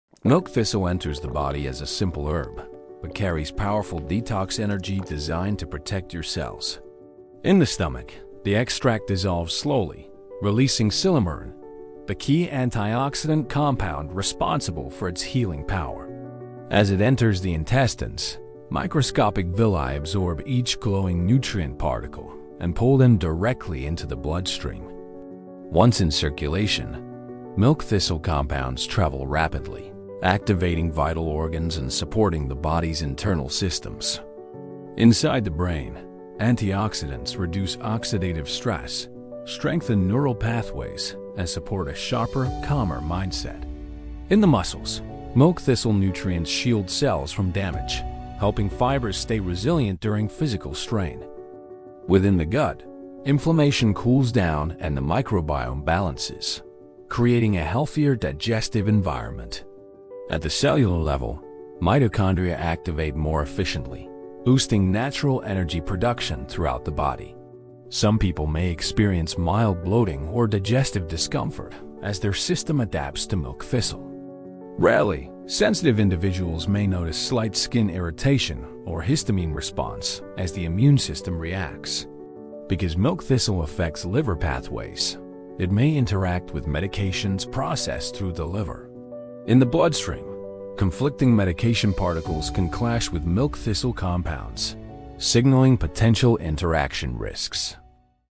Milk Thistle Benefits Explained in 3D Anatomy | Liver Detox, Gut Health & Energy Boost
Discover how Milk Thistle works inside your body using stunning 3D anatomical visuals.
See how it supports liver detox, boosts brain clarity, improves gut health, and enhances cellular energy.
This powerful herb has been used for centuries — now watch what it does inside you in seconds.